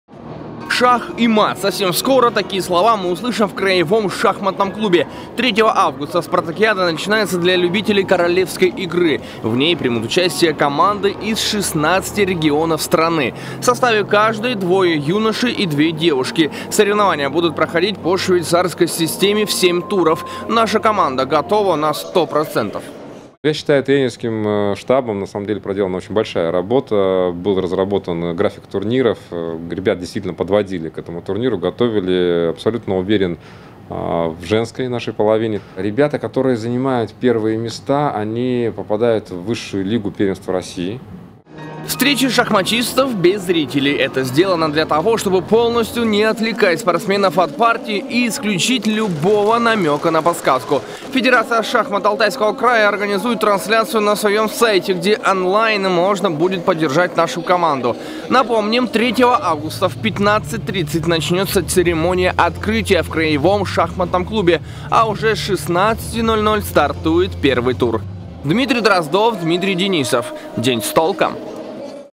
Алтайские шахматисты на 100 % готовы к Спартакиаде учащихся России
Уже в эти выходные шахматисты начнут свои выступления на Спартакиаде учащихся.  Команды прибывают в Барнаул. Состоялось заседание комиссии по допуску участников и все готово к началу соревнований. По какому графику будут проходить игры и почему встречи пройдут без зрителей?